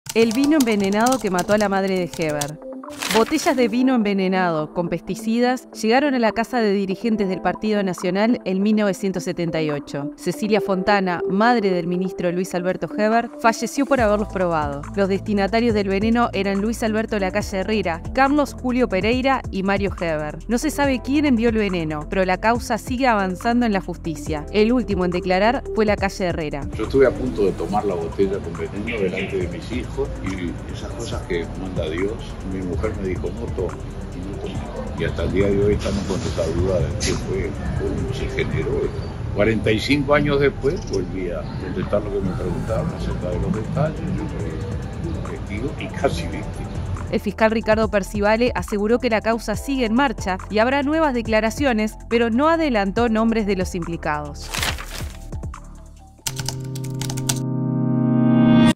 El vino envenenado que mató a la madre del ministro Heber
En 1978 dirigente nacionalistas recibieron botellas de vino envenenadas y Cecilia Fontana, esposa del Mario Heber, lo probó y falleció casi al instante. La causa por su homicidio